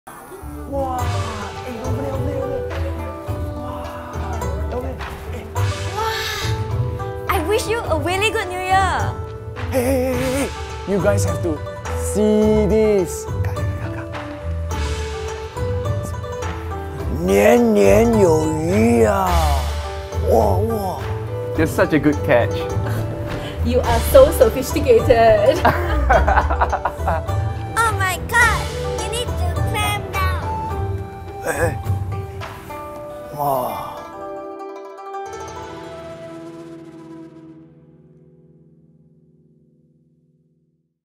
Chinese New Year | S.E.A. Aquarium
Watch a mesmerizing underwater dragon performance at the S.E.A. Aquarium.